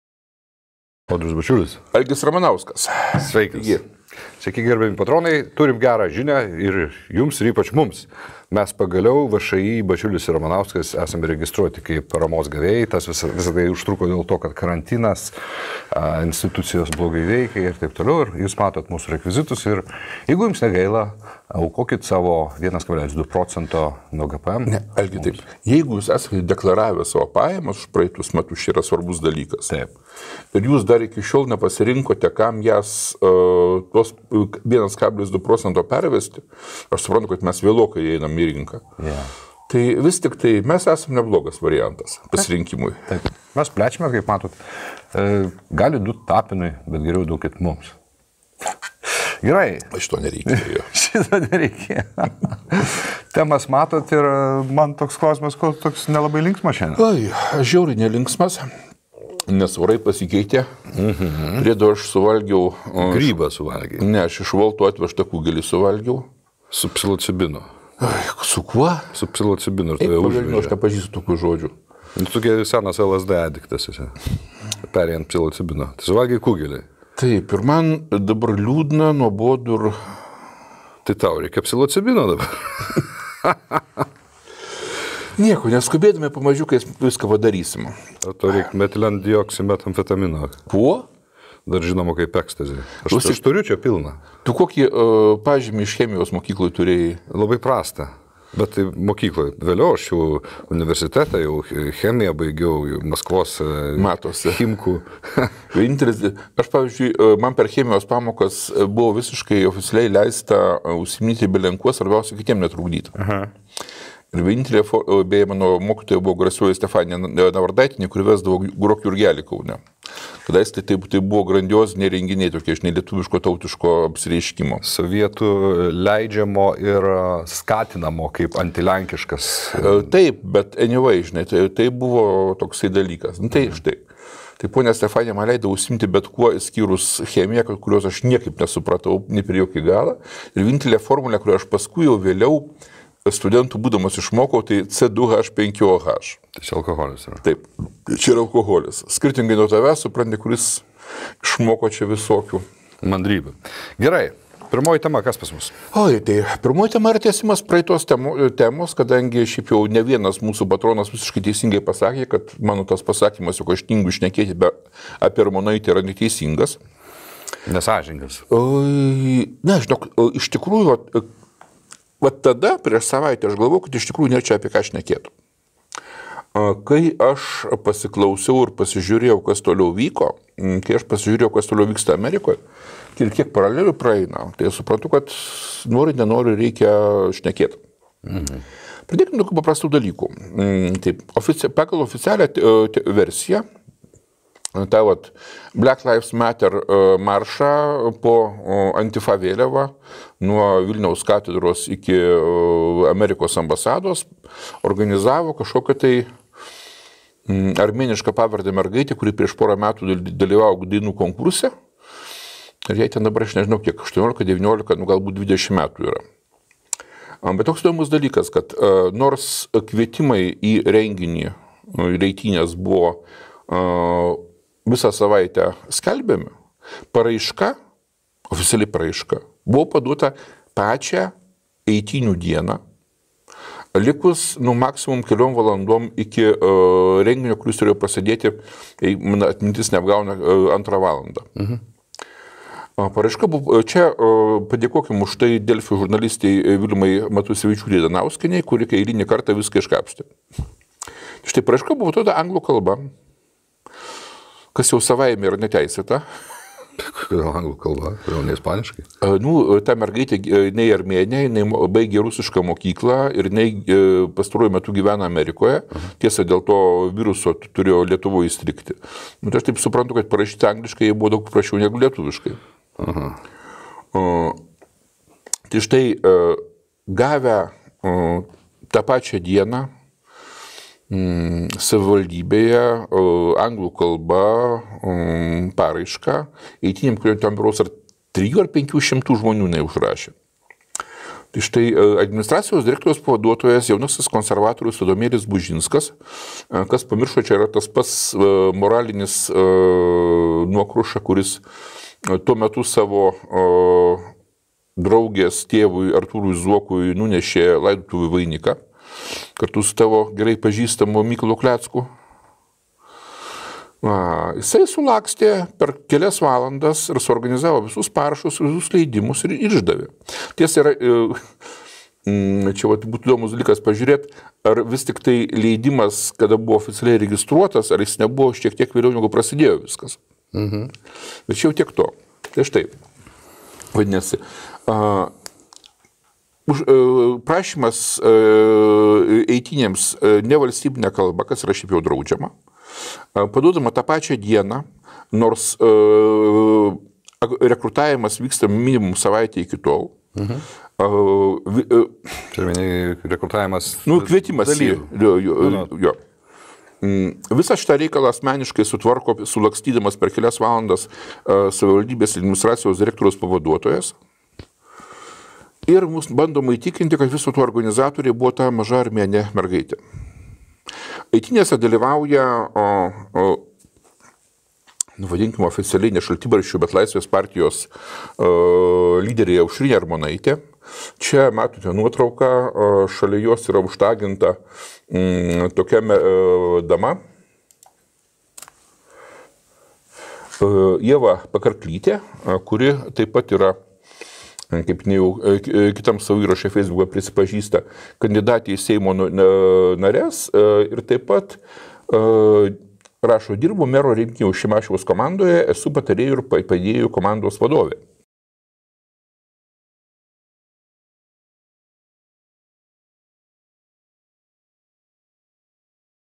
B&R S02E41 20200615 Armonaitės vaikų žygis | Antifa Lietuvoje | Kariškius siundo ant Trampo IŠTR.
PILNO EPIZODO TEMOS:

Armonaitės vaikų žygis
Antifa Lietuvoje 00:39:10
Riaušės JAV 00:49:02

1,2 proc.
VŠĮ “Bačiulis ir Ramanauskas”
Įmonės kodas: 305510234
Šiaulių Bankas. Sąskaitos numeris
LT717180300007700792